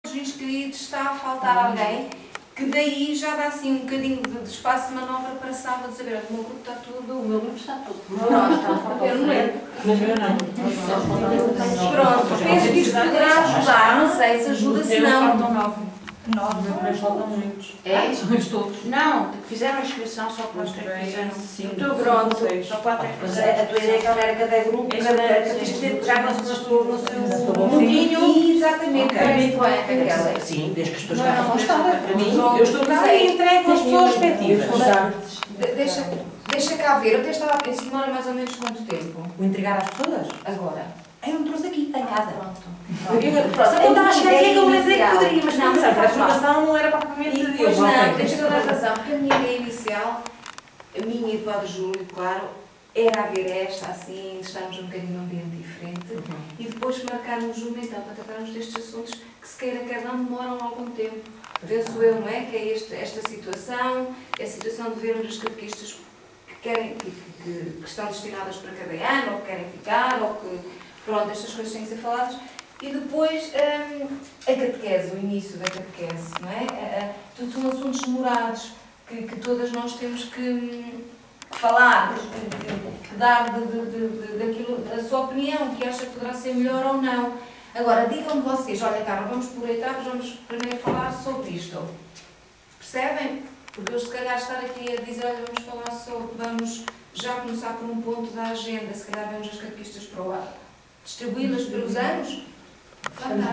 1ª Reunião dos Catequistas da Paróquia de Espinhel para o ano 2014-2015
1ª Reunião dos Catequistas da Paróquia de Espinhel (CENTRO) para o ano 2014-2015 que ocorreu a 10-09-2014 numa sala da Paróquia Espinhelense...